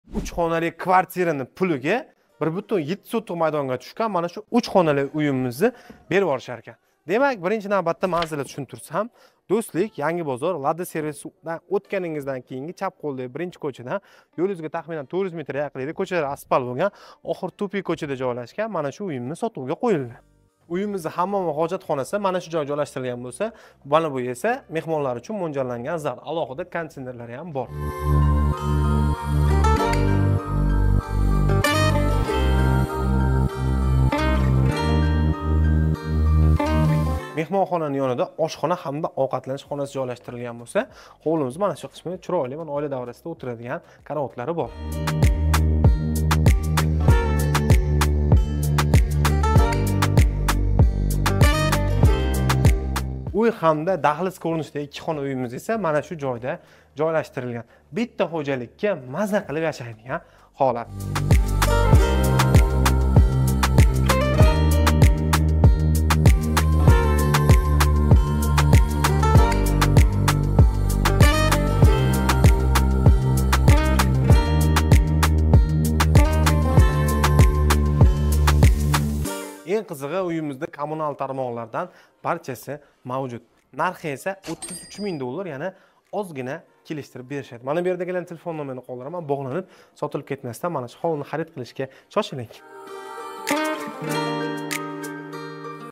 Дўстликда ҳовли сотилади!
(Батафсил маълумотлар видеода)
😎Усмонжон

🏡 1,7 Сотиҳ
🔢 3 хонали
🔥💧⚡️Газ, сув, свет Канализация бор
✅ Хужжатлари жойида
💸 33.000 озгина ками бор

📭 Дўстлик Lada сервиздан ўтганда чап қўлдаги 1-кўчада